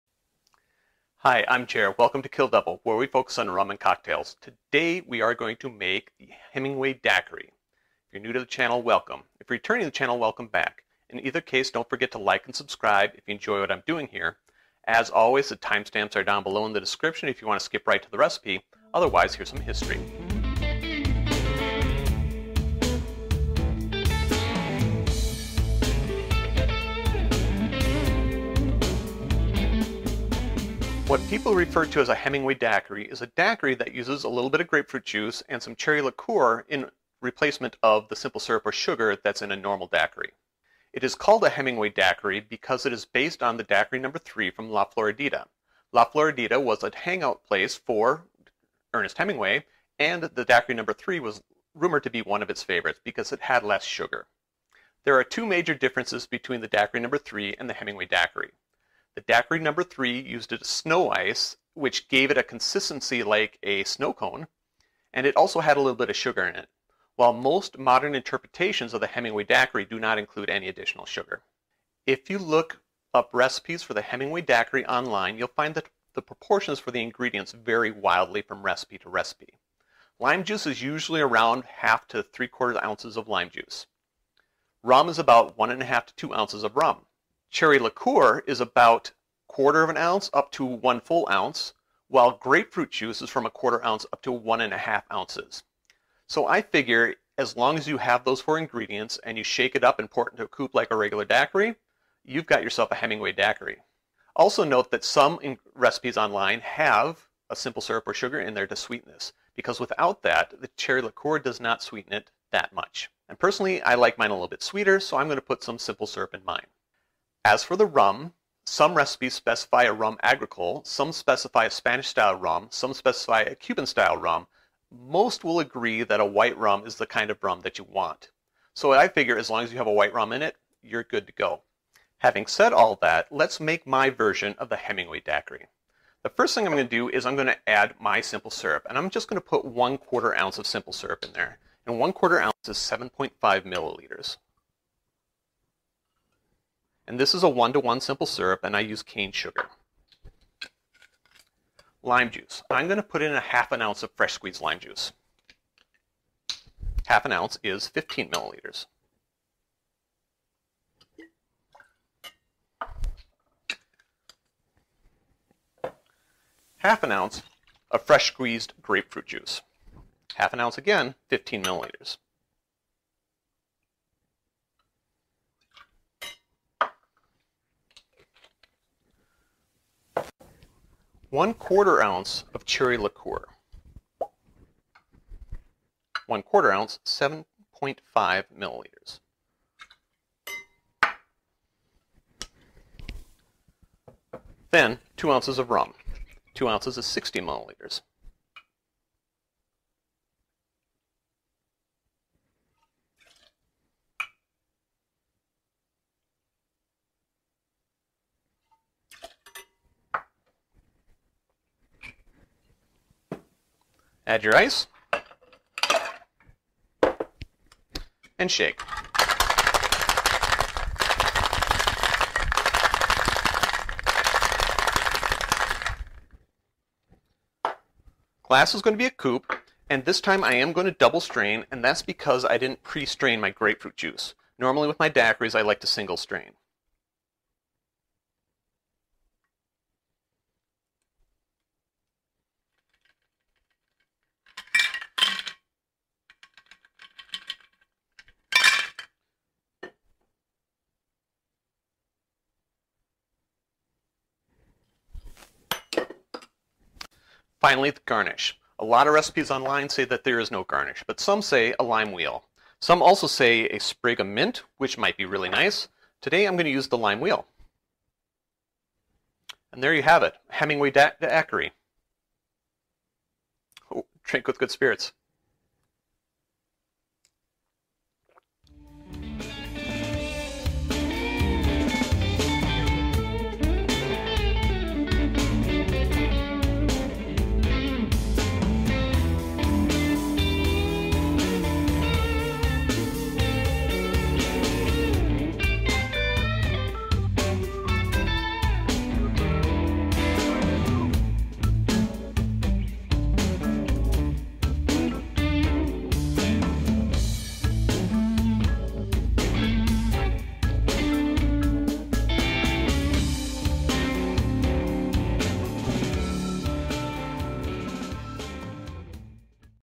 How to Make a Hemingway Daiquiri - Rum Cocktail - Cocktails At Home
If you want to learn about and make a Hemingway Daiquiri, then watch this video!

NEED GEAR?
Cocktail Shakers
All-in-One Cocktail Bar Kits

   T-shirts available! DM me through social media links!

TIMESTAMPS
0:00 Intro
0:20 Title
0:30 Background
1:56 Rum Notes
2:11 How to Make
4:40 Recipe
5:07 Credits

RECIPE

Hemingway Daiquiri
2-oz (60 mL) white rum
0.25-oz (7.5 mL) maraschino liqueur
0.5-oz (15 mL) lime juice
0.5-oz (15 mL) grapefruit juice
0.25-oz (7.5 mL) simple syrup (1:1)

Garnish: Lime wheel
Glass: Coupe

Instructions: Shake ingredients. Strain into glass. Garnish.

Version created by: Jer Anderson.

CREDITS
Recorded by: J. Anderson
Edited by: J. Anderson
Graphic Design by: Peng Wu